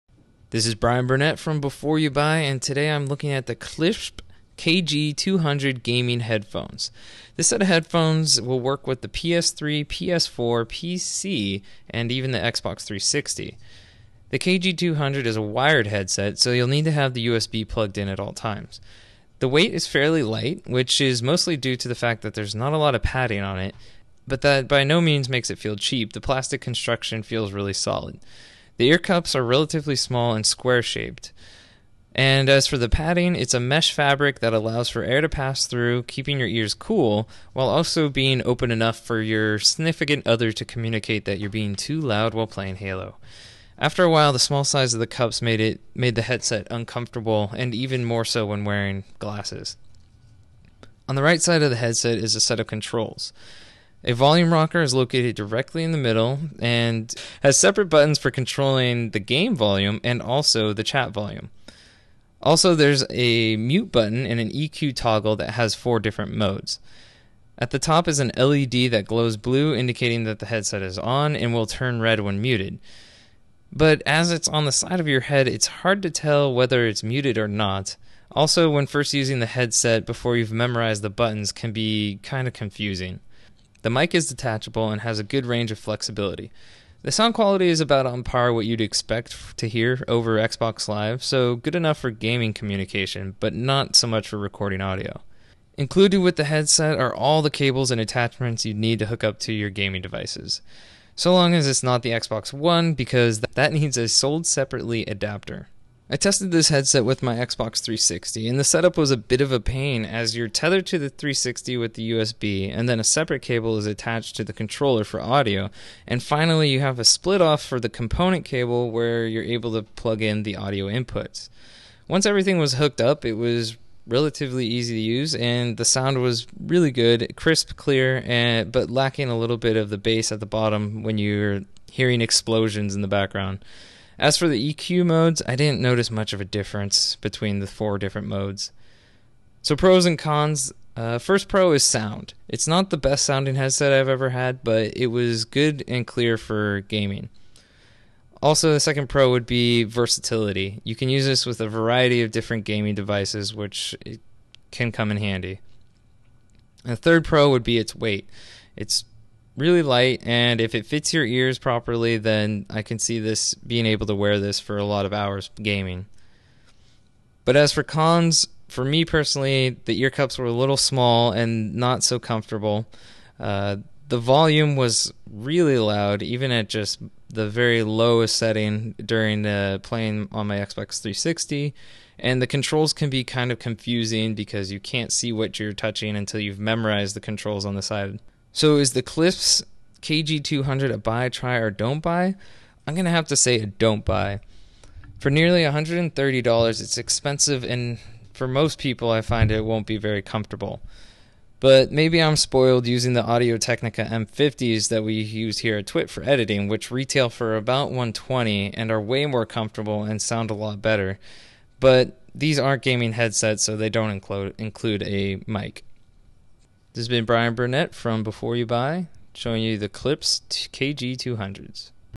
Klipsch KG-200 Gaming Headphones
Bryan Burnett from BYB reviews the Klipsch KG-200 which is designed for use with PlayStation 4, PlayStation 3, Xbox 360 and PCs.

Pro
- Sound
- Versatile
- Weight

Con
- Comfort
- Volume
- Expensive

Don't Buy!